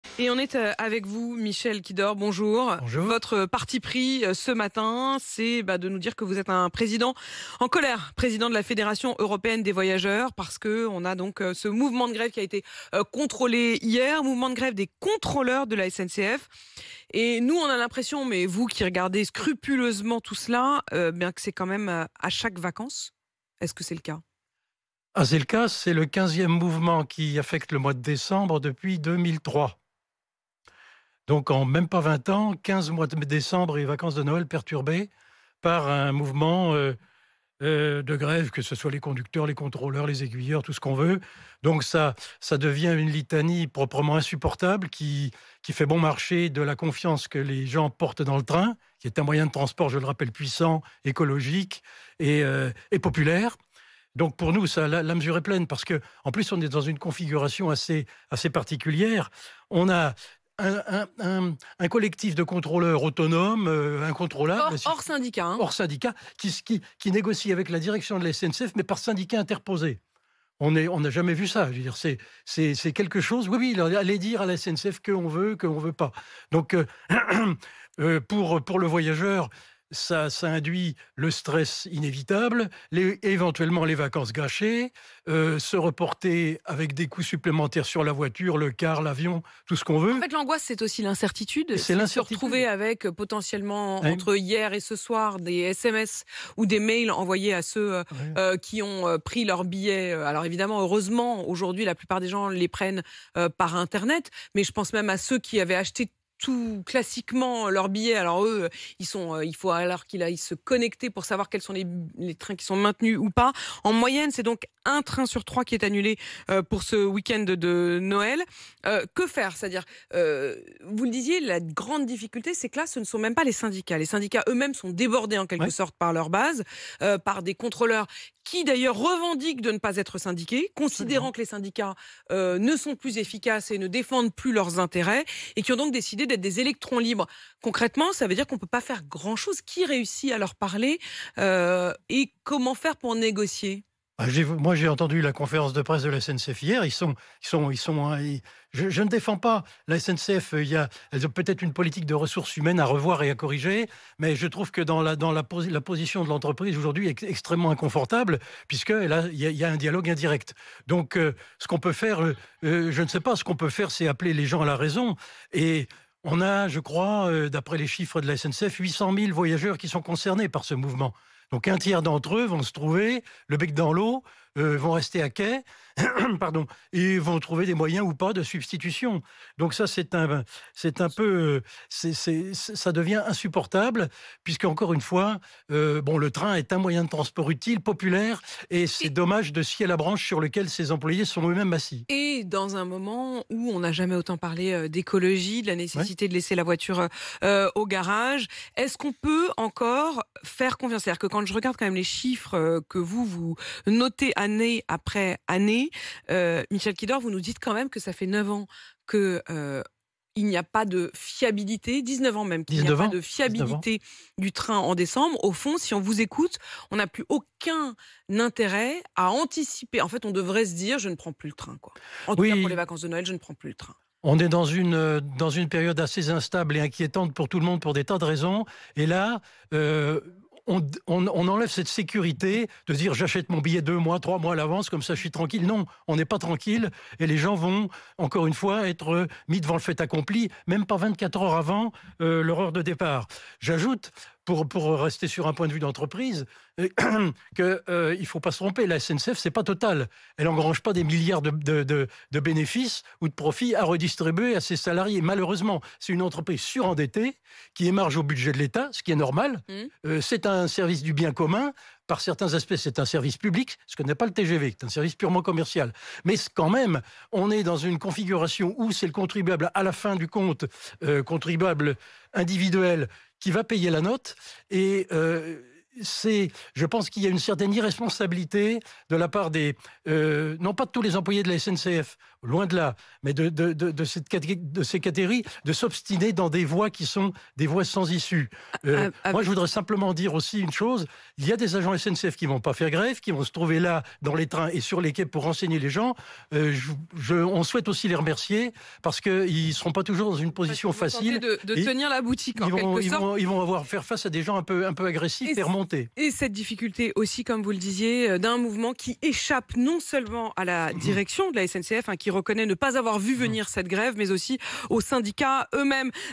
Grève SNCF pour Noël : "ça devient une litanie insupportable"
Michel Quidort, président de la Fédération européenne des voyageurs, explique sa colère face au mouvement de grève à la SNCF pour Noël.
"C'est le 15e mouvement qui affecte le mois de décembre depuis 2003, cela devient une litanie insupportable", s’agace-t-il.